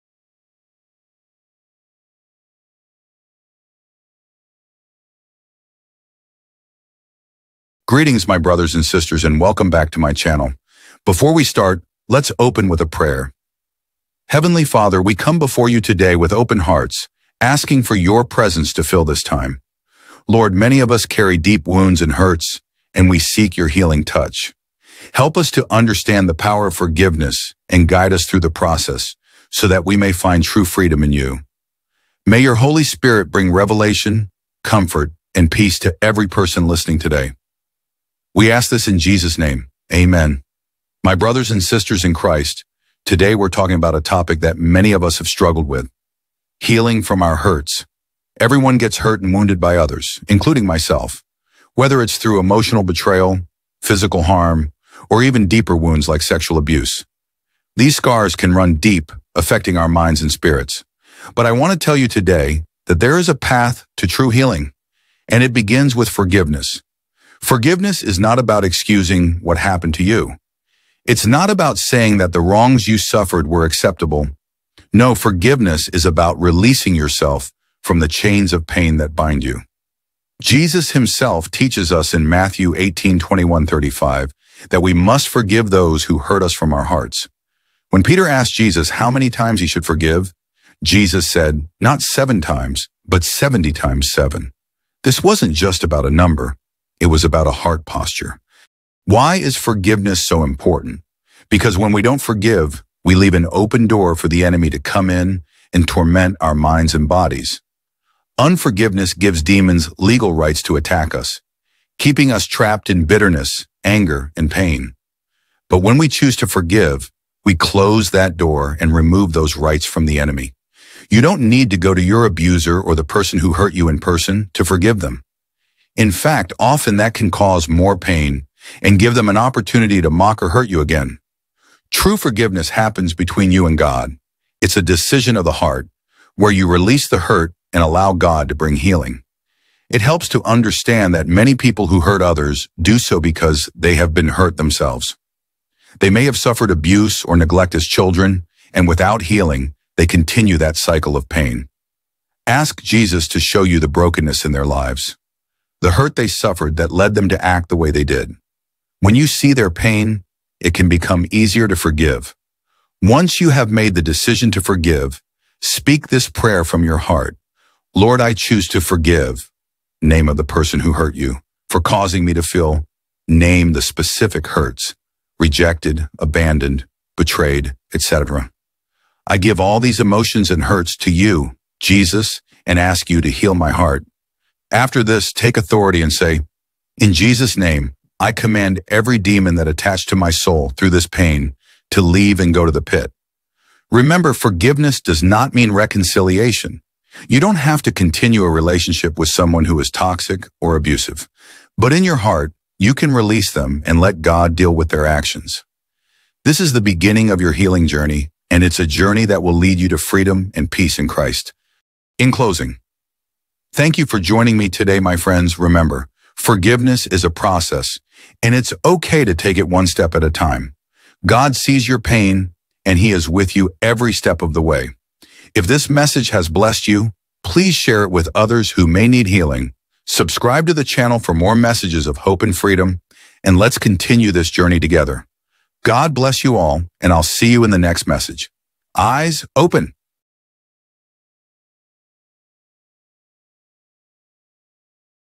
Forgiveness 2024, do it now!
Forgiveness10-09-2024
 let’s open with a prayer: Heavenly Father, we come before You today with open hearts, asking for Your presence to fill this time. Lord, many of us carry deep wounds and hurts, and we seek Your healing touch. Help us to understand the power of forgiveness and guide us through the process, so that we may find true freedom in You. May Your Holy Spirit bring revelation, comfort, and peace to every person listening today. We ask this in Jesus' name. Amen.